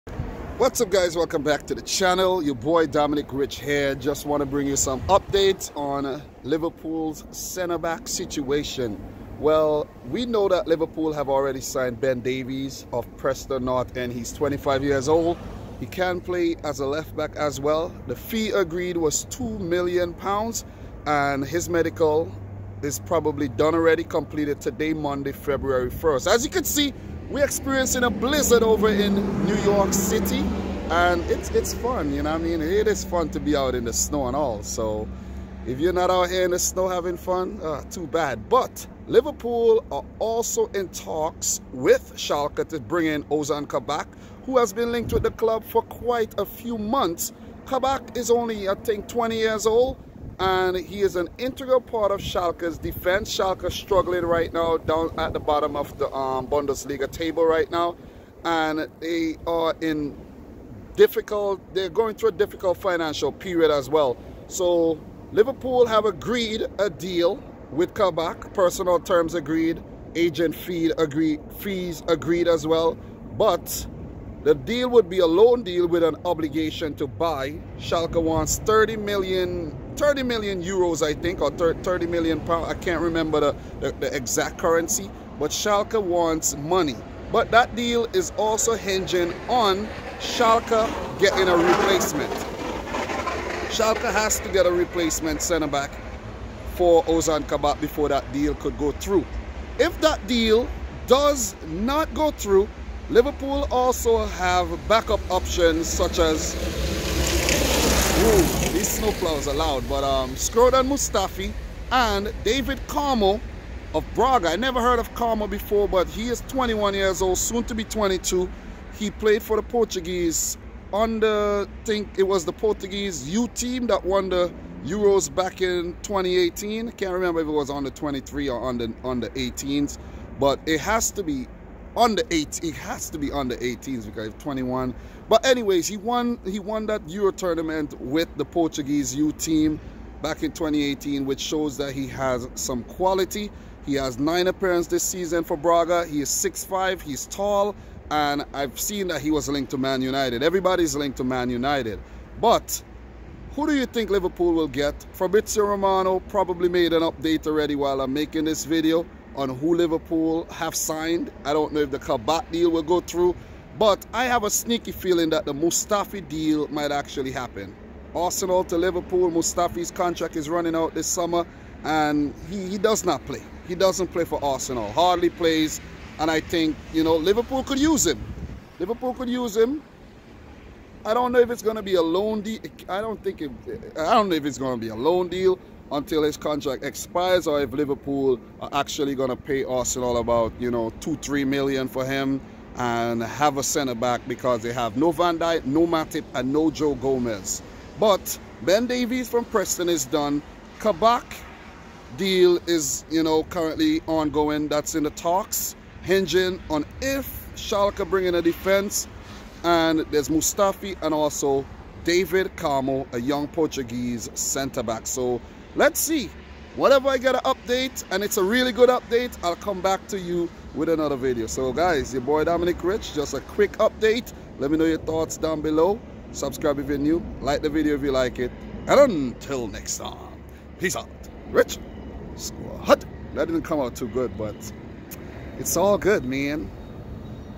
Liverpool Agrees Ozan Kabak Deal! David Carmo & Shkrodran Mustafi Are Possible Options
Liverpool Agrees Ozan Kabak Deal!  David Carmo & Skrodran Mustafi Are Possible Options

Dominic